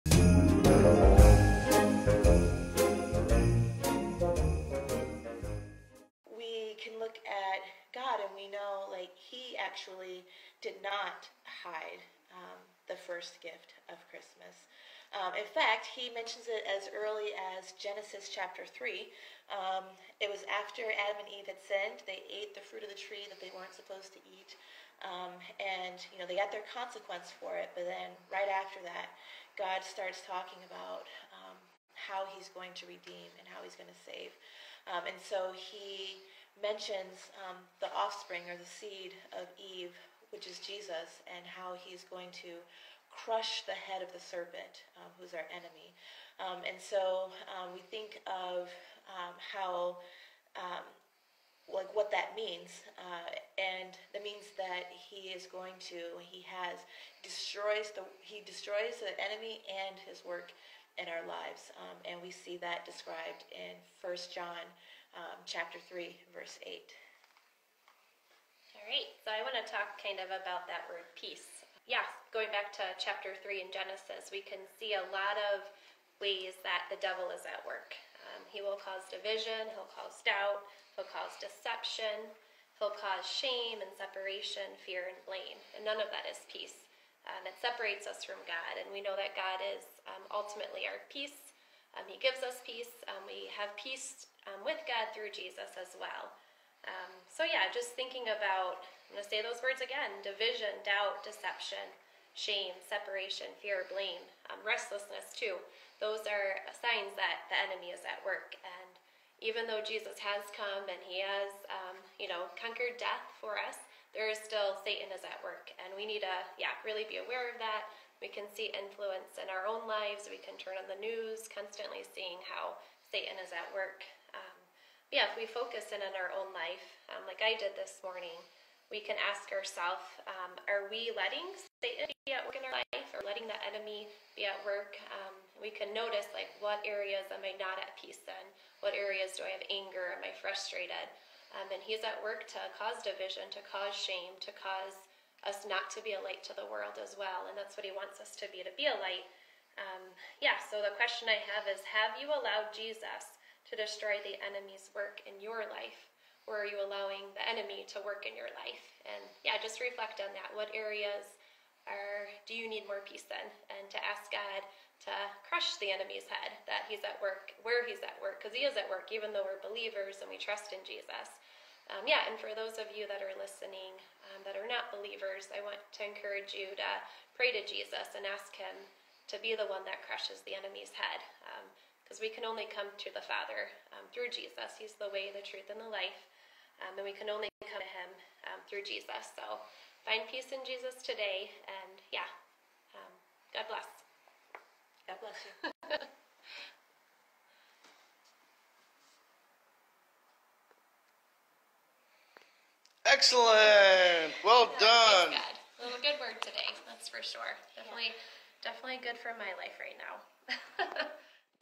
Victory Over Satan, Advent 8, Amanda & Chrisara
Meet Chrisara and Amanda. Today for the 2nd Sunday in Advent (Day 8, Peace) they reflect on the thematic elements of Pastor Andrew's excellent sermon at Fairlane Alliance Church. Hear how the capture them. They especially focus on God's messianic declaration in the Torah, the Book of Genesis 3:15 in which He said that the seed of the woman (the Messiah) would one day crush the head of the serpent (Satan); and millennia later in the New Testament, the Apostle John's statement that Jesus came to destroy the works of the devil in fulfillment of that prophecy (1 John 3:8). For those who walk with Jesus, these and many others offer hopeful implications for victory over the evil one.